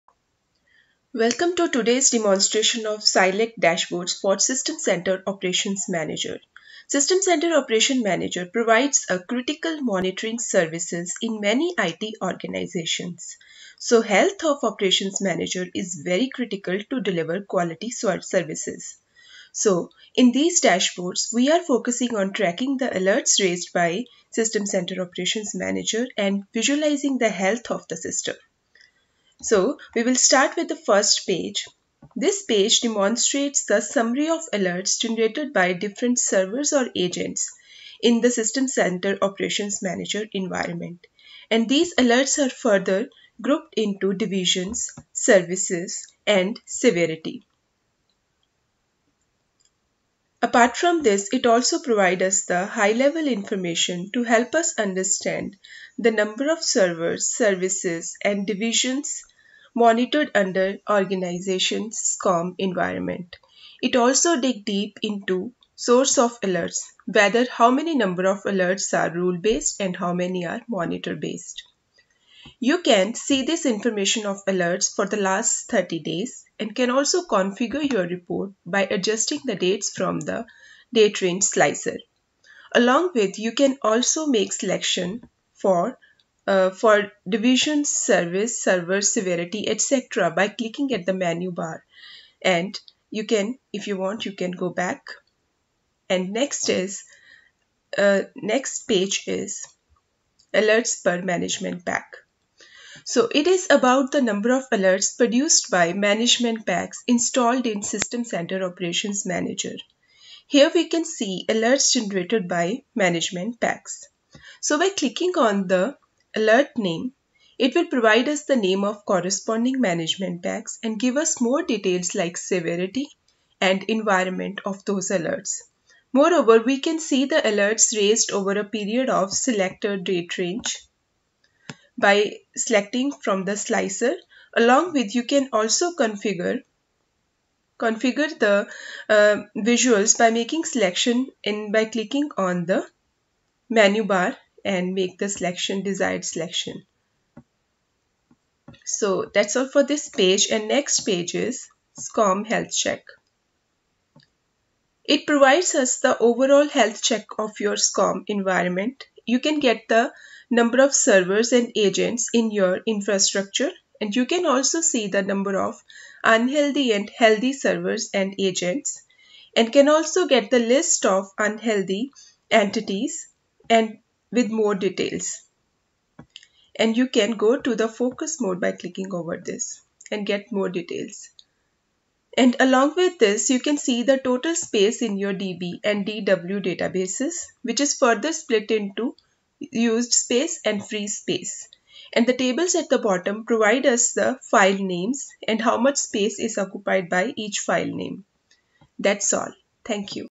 Silect Dashboards for SCOM v2
Silect’s new Dashboards for SCOM leverages the power of Power BI to critical information about the state of your IT infrastructure. See summarized information about Alerts generated by System Center Operations Manager then drill down into the details. See alerts broken down by location, by division and by service. Learn which management packs are creating the most alerts and also get an overview of the health of your SCOM environment.